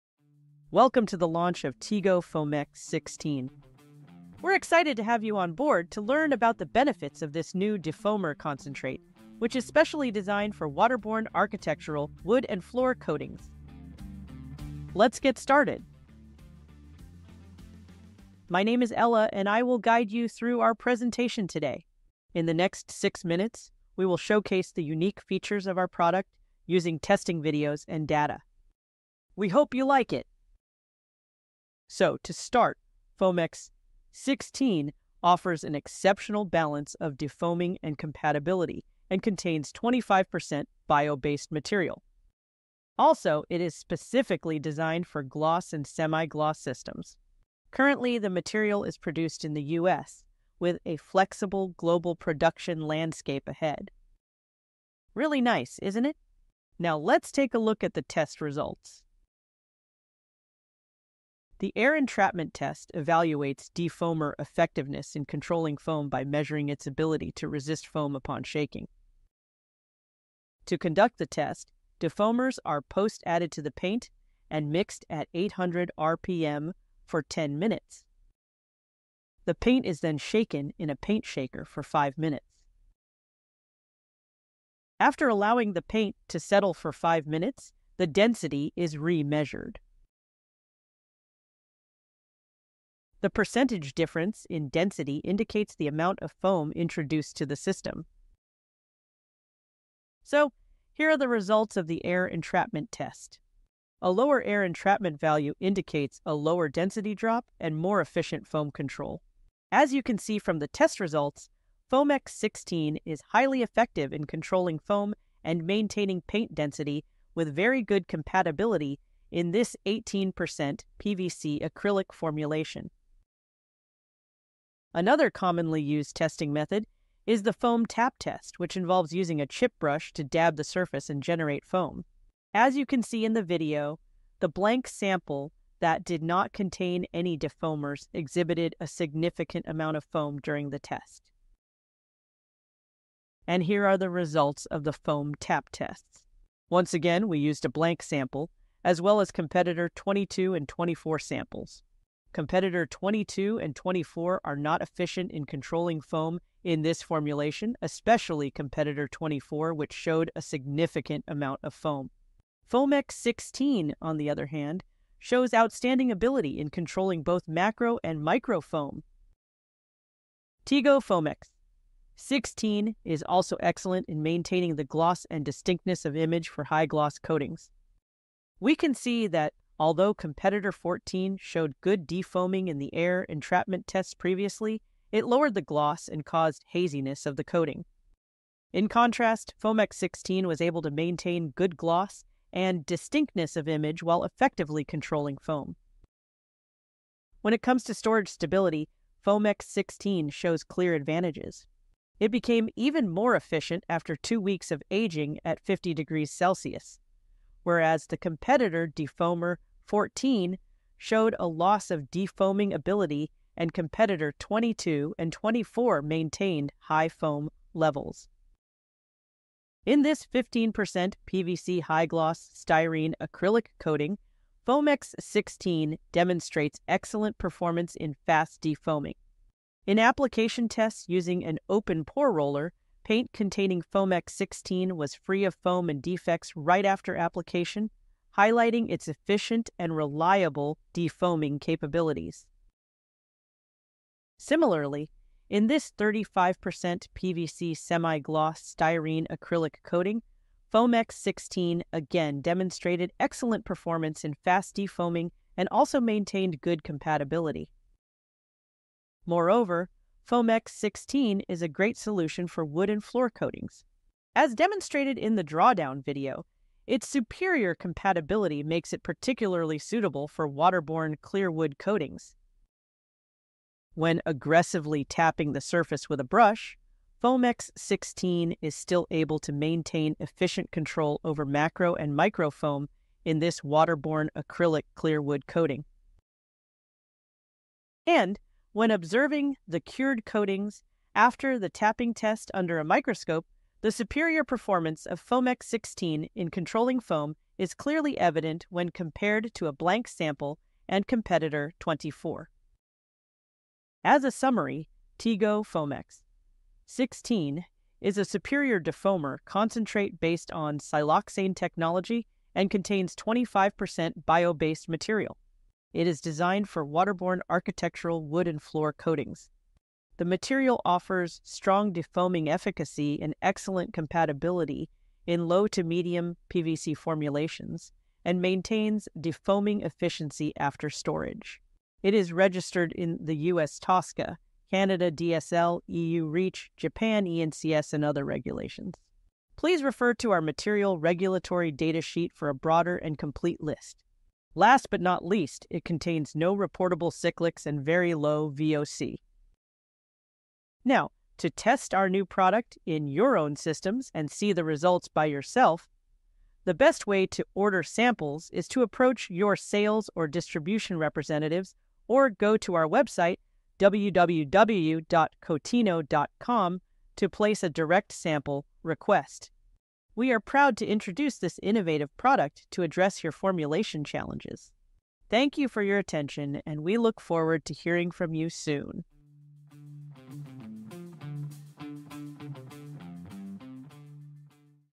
Introduction of TEGO® Foamex 16 | Evonik
TEGO® Foamex 16 is Evonik's latest defoamer concentrate for waterborne architectural, wood, and floor coatings. This partially renewable siloxane-based defoamer offers exceptional performance with an outstanding sustainability profile. The video covers the various advantages of TEGO® Foamex 16 and explains how it can future-proof your formulation.